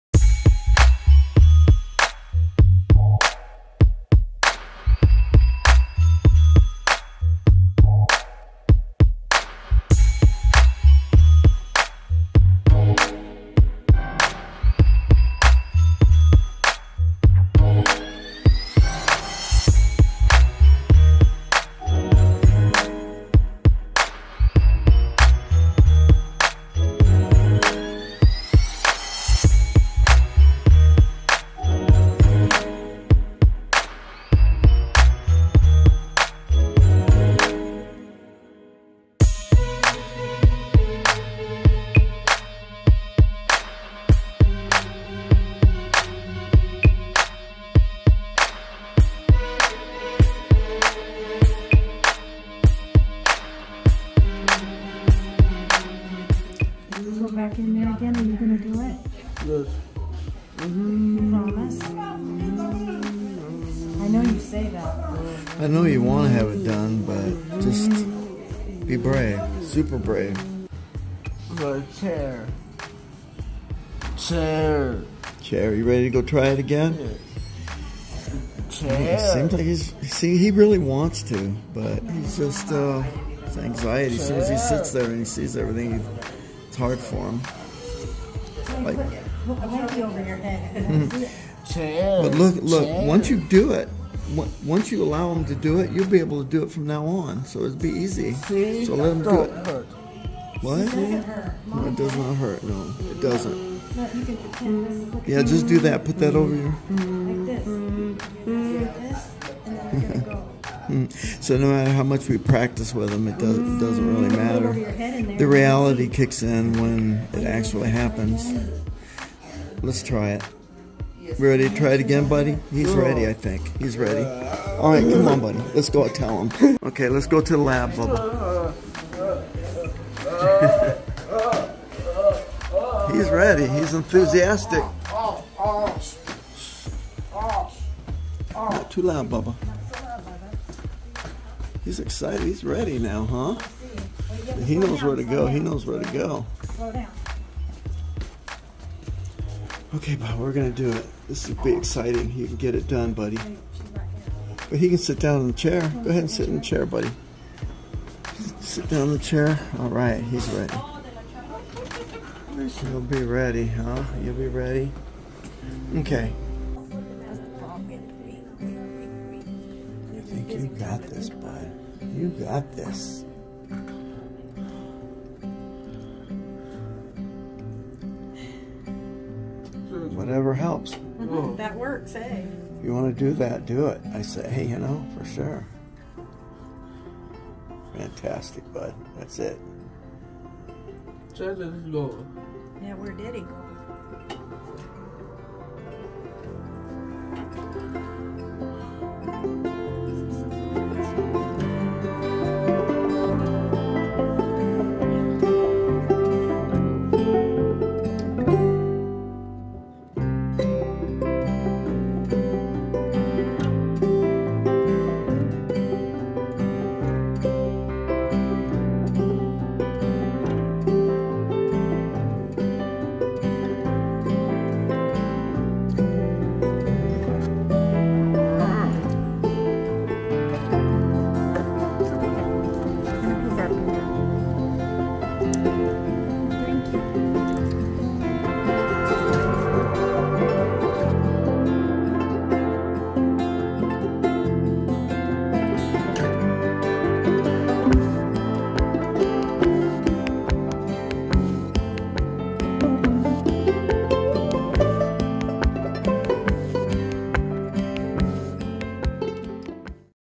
Blood Draw Recap
ust like JJ's shirt says, not today.

We are back to square one for a blood draw and Dentist.

The doctor is going to help us get a dentist at Phoenix Children's Hospital and the plan is to get bloodwork while he is sedated for his dental work.

After it was all over, JJ suddenly got very emotional.

Could it be that he feels guilty for not being brave enough to get the blood draw, or maybe is it's an effect of the meds he took to calm him?

It broke our heart to see him this way. 💔

Sweet boy!